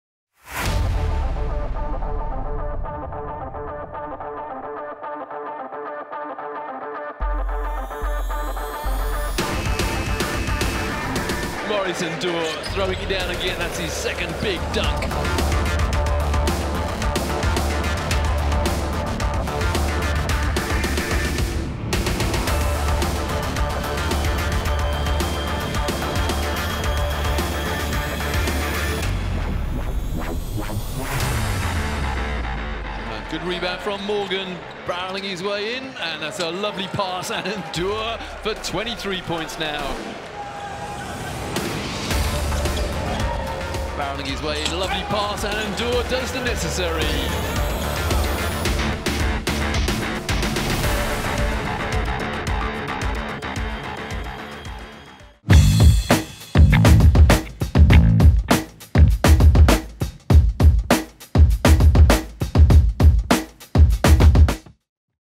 7DAYS EuroCup Regular Season Round 1 MVP: Maurice Ndour, UNICS Kazan
The opening round of the 7DAYS EuroCup Regular Season had some great performances, but no player shined brighter than UNICS Kazan forward Maurice Ndour.